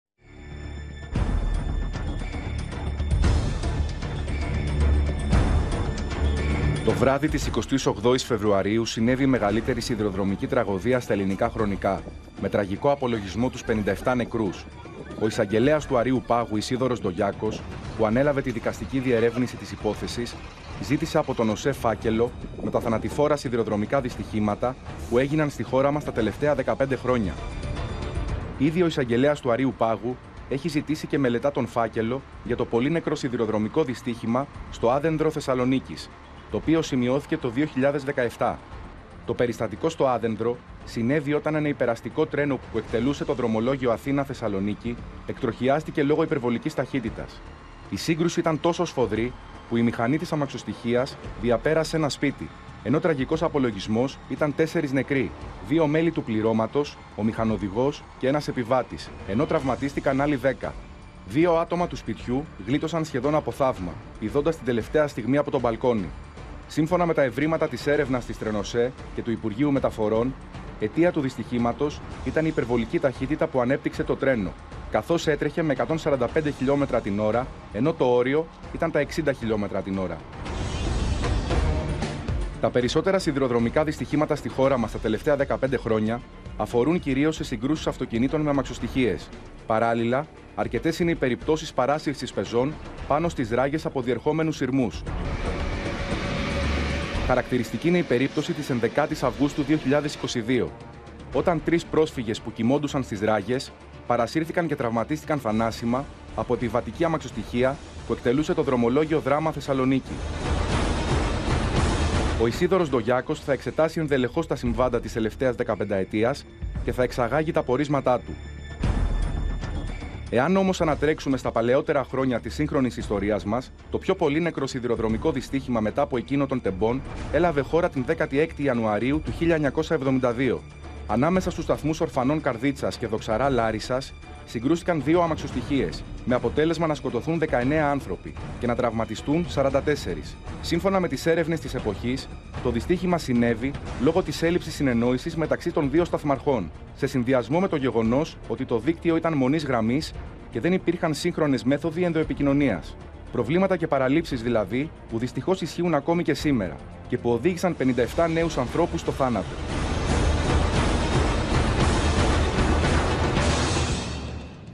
Ποια είναι τα σιδηροδρομικά δυστυχήματα της 15ετίας που θα εξετάσει ο Ντογιάκος | OPEN TV
Γίνετε μέλος στο κανάλι του OPEN για να αποκτήσετε πρόσβαση σε προνόμια:
Ανοίγουν φάκελοι και «στόματα» σε βάθος 15ετίας. Ποια είναι τα σιδηροδρομικά δυστυχήματα που θα εξετάσει ο Ντογιάκος.